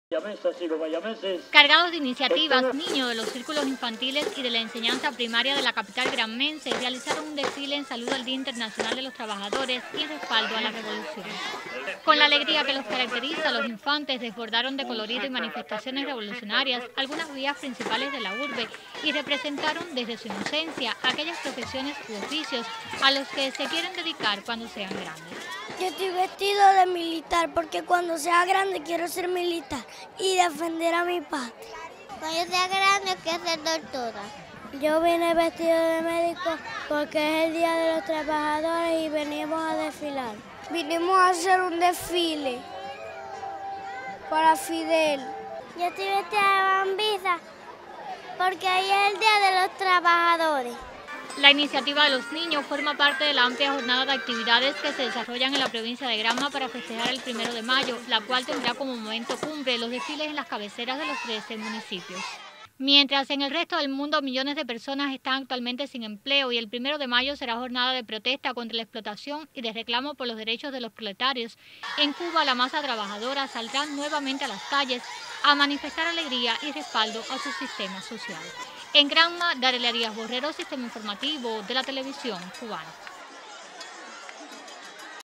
Realizan en Granma desfile infantil en saludo al Primero de Mayo
Caracterizado por la alegría y el colorido se desarrolló, este viernes, en Bayamo un desfile infantil en saludo al Primero de Mayo, Día Internacional de los Trabajadores.

En la actividad, que tuvo como punto de partida la icónica plaza de la Patria, única del país con ese nombre, participaron infantes de los círculos infantiles y de la enseñanza primaria.